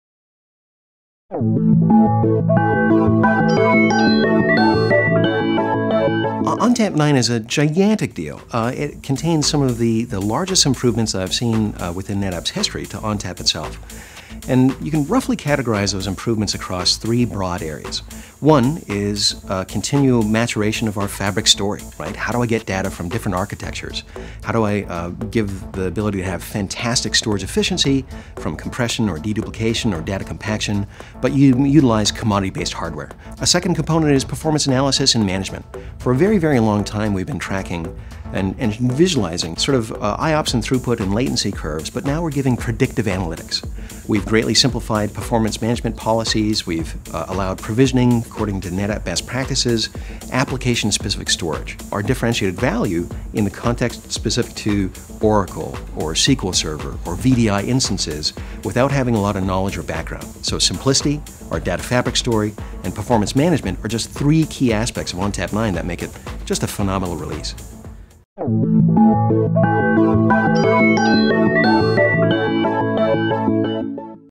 Insight 2016 Sneak Peek: Why ONTAP 9 is a big deal
The new major release of ONTAP software is ONTAP 9.  Here's a teaser of what you'll learn at NetApp Insight 2016.